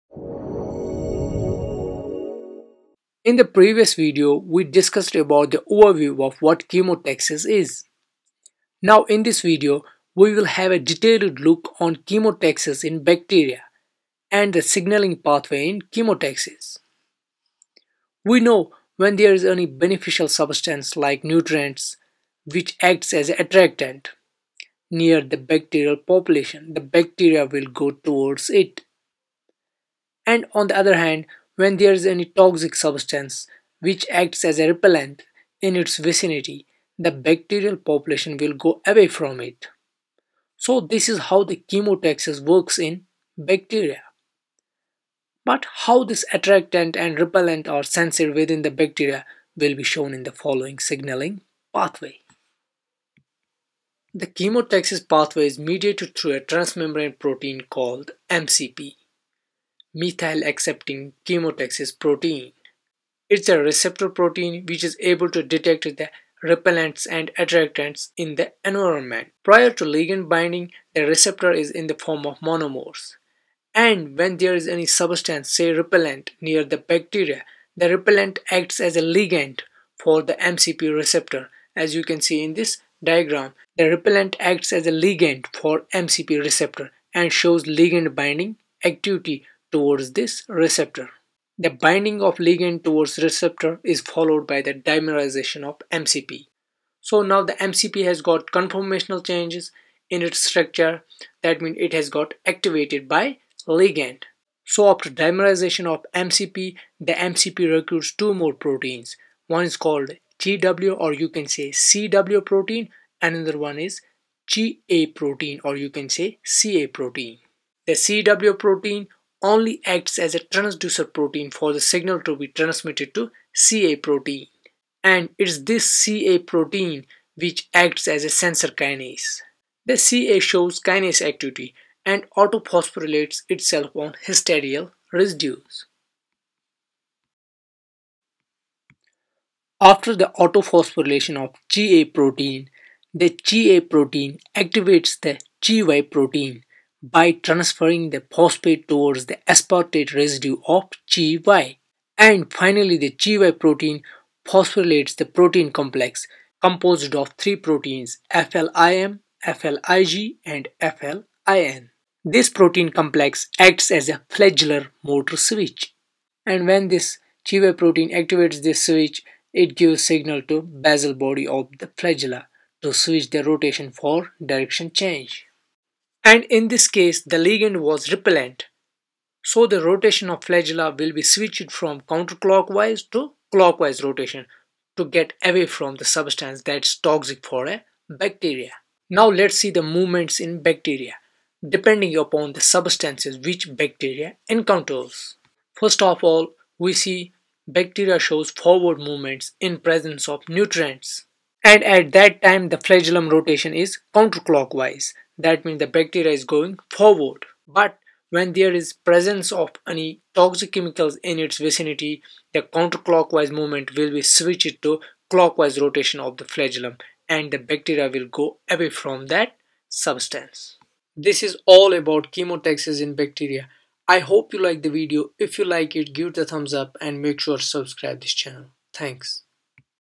Chemotaxis | Signaling Pathway in Bacteria
Chemotaxis is the movement of an organism in response to a chemical stimulus. Somatic cells, bacteria, and other single-cell or multicellular organisms direct their movements according to certain chemicals in their environment.